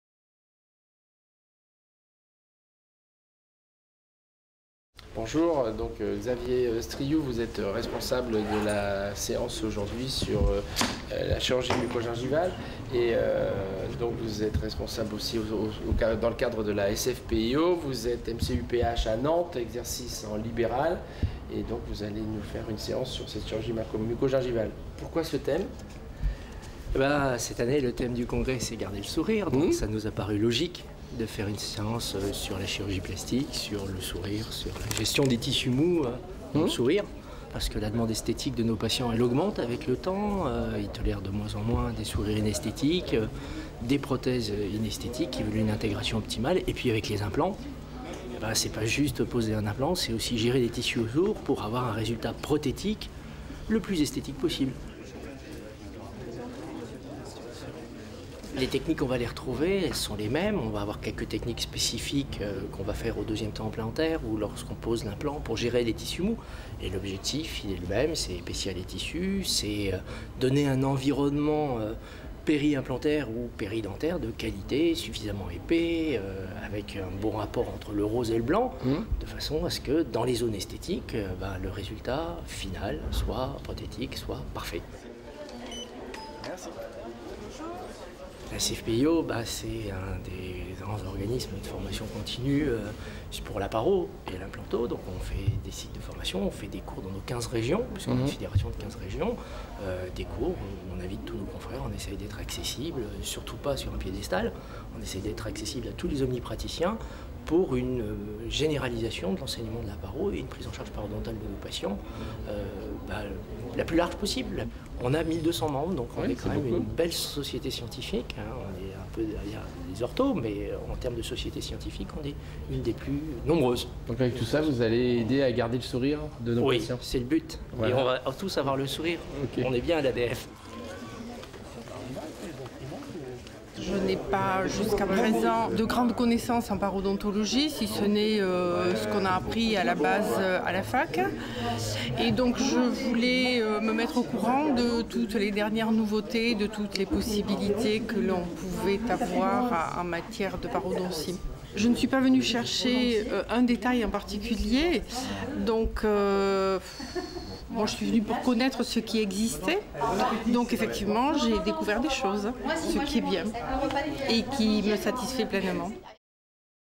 Congrès ADF 2012 : 29/11 : séance SFPIO : C67 La chirurgie muco-gingivale : Xavier Struillou
En direct des salles de conférences : séance SFPIO : Conférence C67 La chirurgie muco-gingivale Questions au Dr Xavier Struillou et aux congressistes assistant à la séance